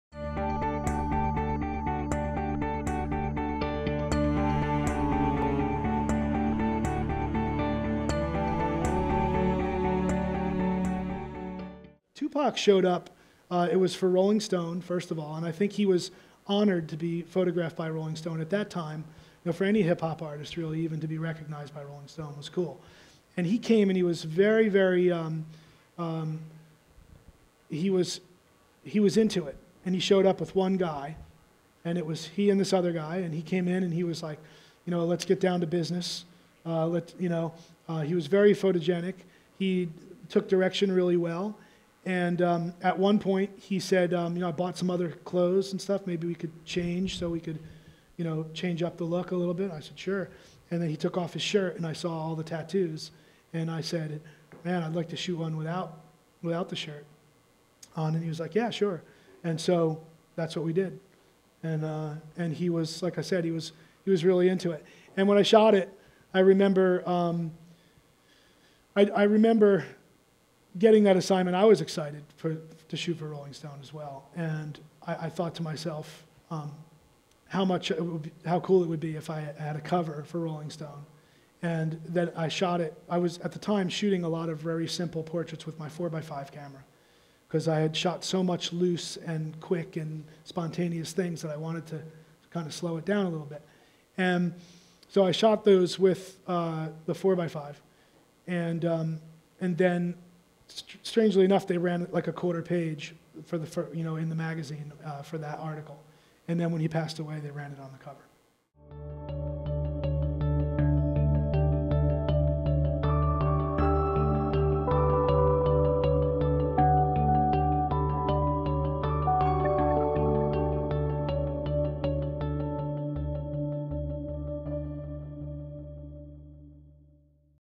Danny Clinch on Tupac Shakur
Danny Clinch discusses photographing Tupac Shakur for the Annenberg Space for Photography's Iris Nights lecture series.

Interested?